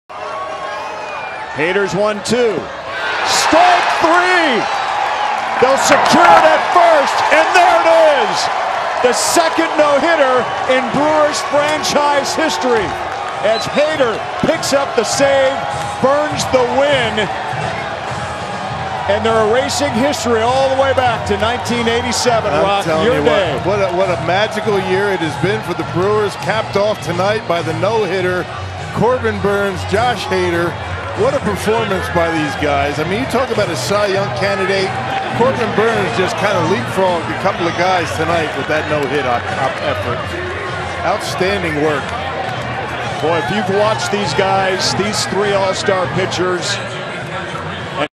Hader completes the 9th no hitter of the 2021 season! 2nd combined of the year!
Join Über Alles